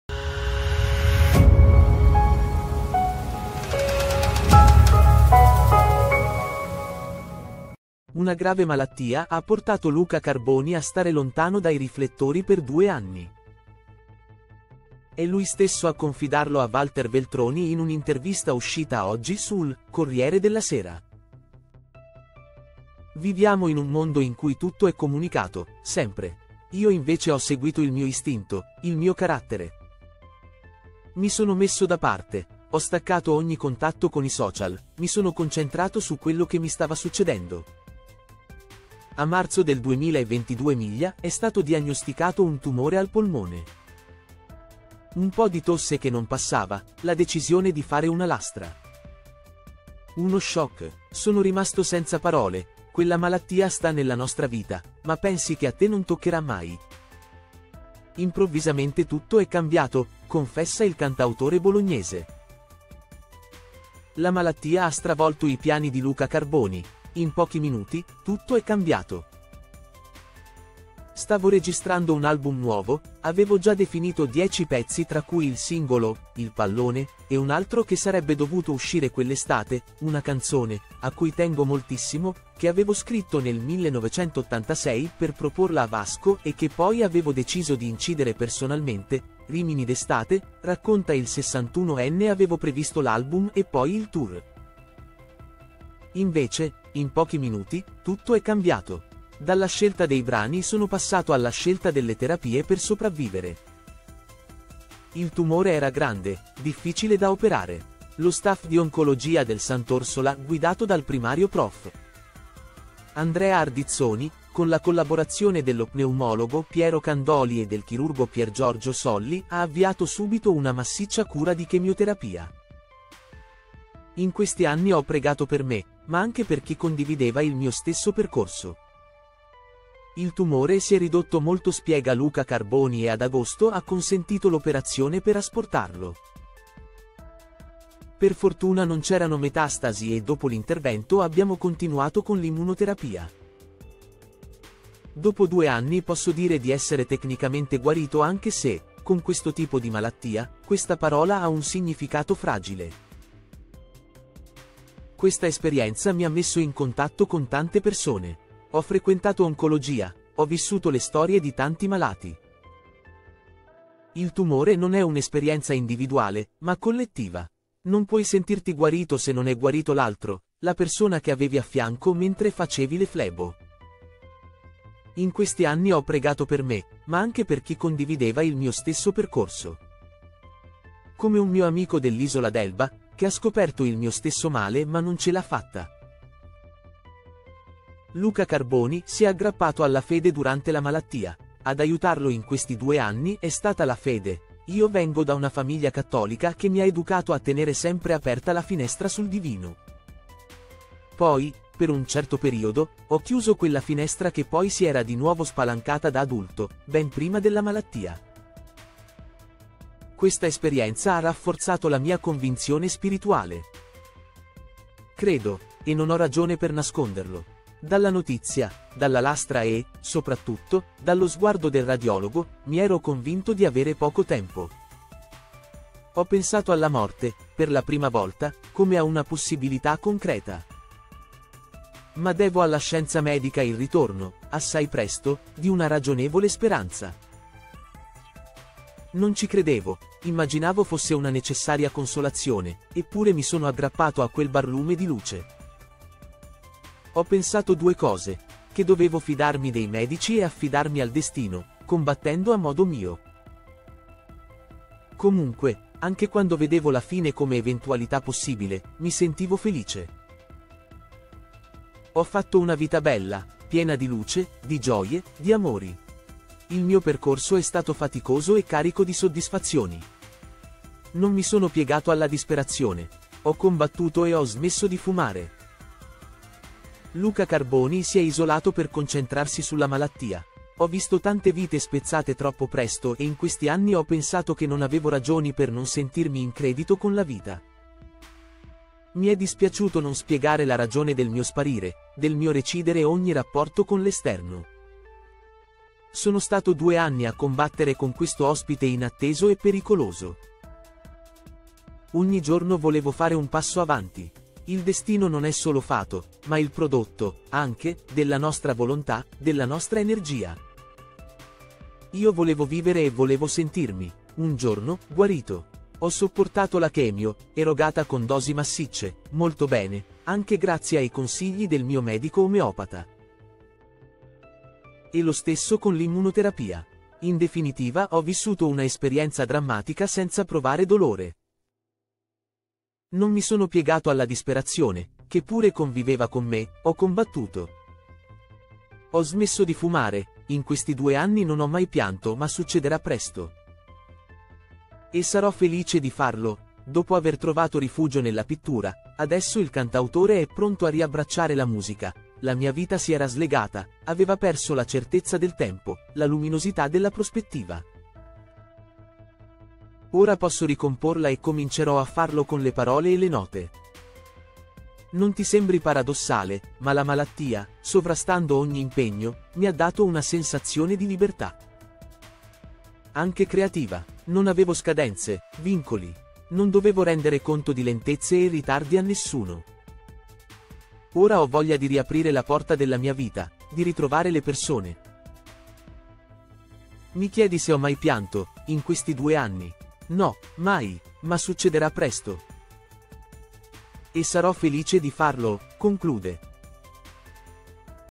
Luca Carboni racconta la malattia “Ho avuto un tumore ai polmoni, mai pianto in questi 2 anni”
Una grave malattia ha portato Luca Carboni a stare lontano dai riflettori per due anni. È lui stesso a confidarlo a Walter Veltroni in un’intervista uscita oggi sul “Corriere della Sera”. “Viviamo in un mondo in cui tutto è comunicato, sempre. Io invece ho seguito il mio istinto, il mio carattere. Mi sono messo da parte, ho staccato ogni contatto con i social, mi sono concentrato su quello che mi stava succedendo. A marzo del 2022 mi è stato diagnosticato un tumore al polmone. Un po’ di tosse che non passava, la decisione di fare una lastra. Uno choc. Sono rimasto senza parole, quella malattia sta nella nostra vita, ma pensi che a te non toccherà mai. Improvvisamente tutto è cambiato”, confessa il cantautore bolognese.